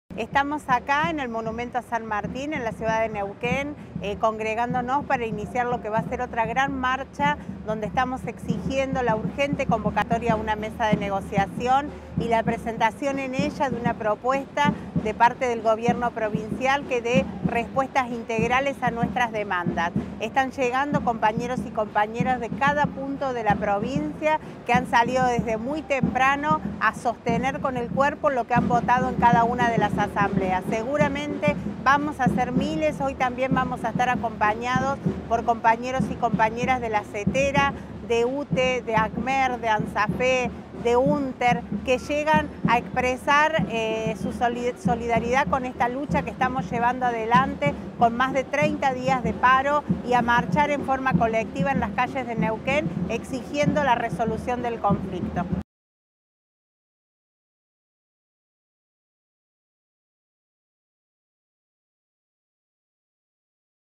Somos miles! Aten en la calle exige urgente Convocatoria a Mesa de Negociación!
Compañerxs de toda la Provincia exigen en la Calle la Urgente Convocatoria a Mesa de Negociación! Gutierrez debe convocar Ya!!aten